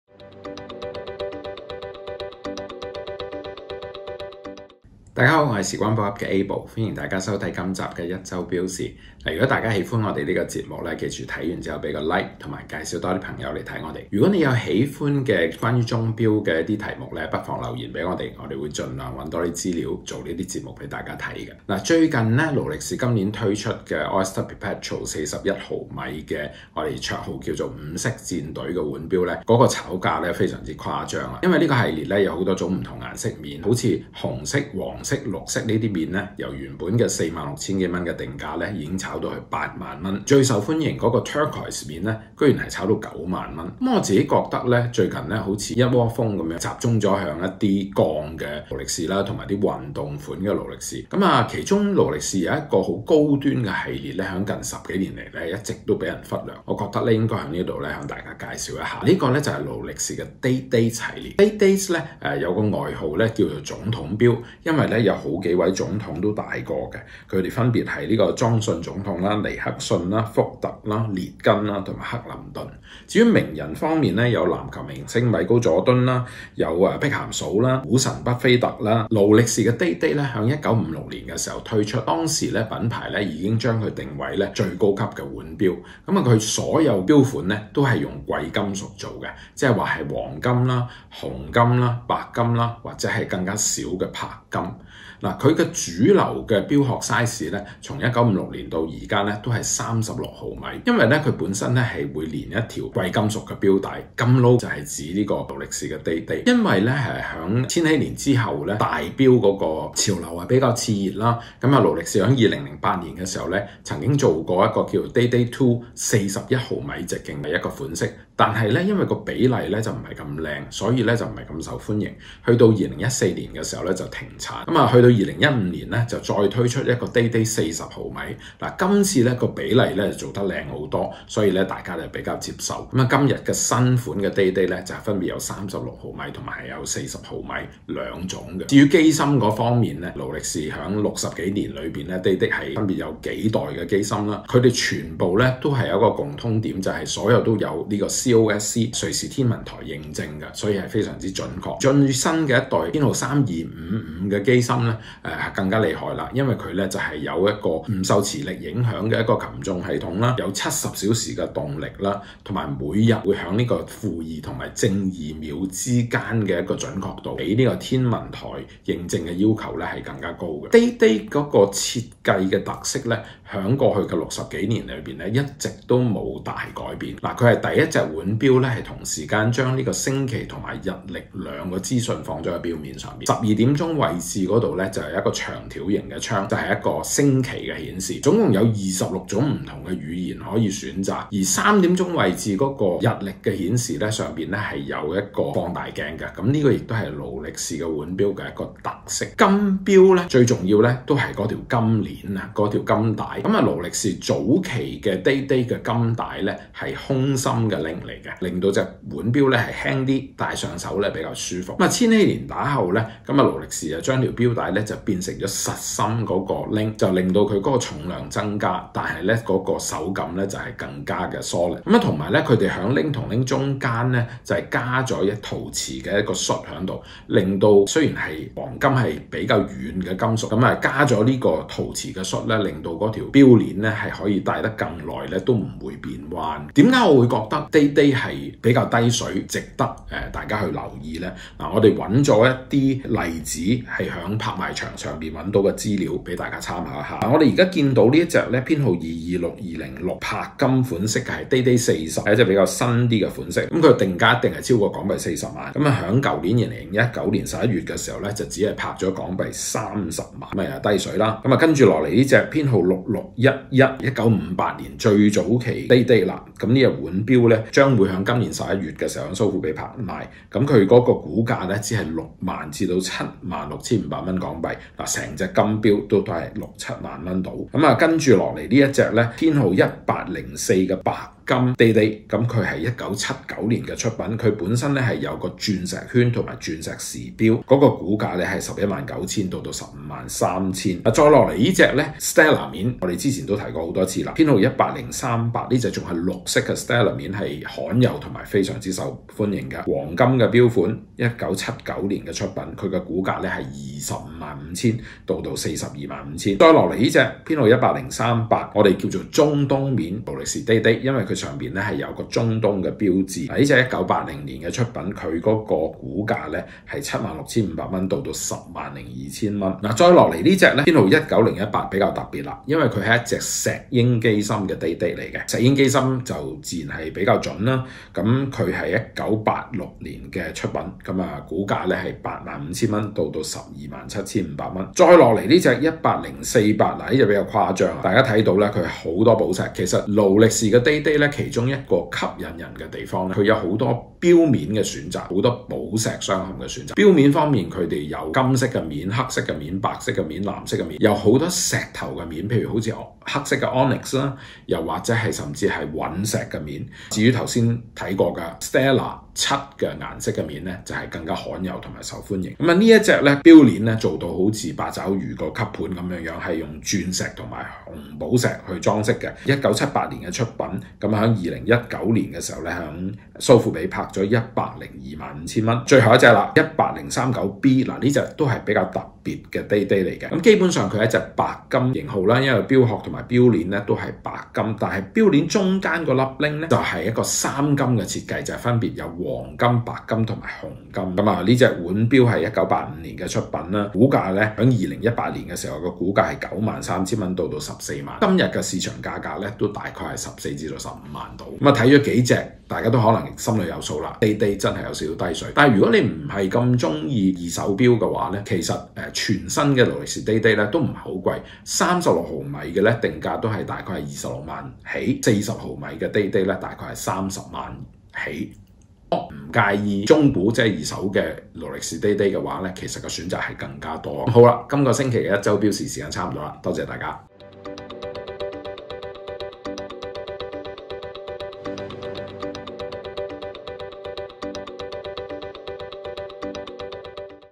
【一周表事】邊隻「撈」最低水？ Day-Date 殊不簡單的「星期」變法！
勞力士不是只有Daytona，
要尋寶，認住這一彎漂亮的星期顯示。

夠經典，60多年帥氣外型如一。
夠貼地，星期顯示就用您最熟悉的文字。
夠好玩，顏色材質任君挑選。

最重要是——低水。
與其隨波逐流，盲目以天價搶購，
不如用心發掘這些
潛力無限的佳品！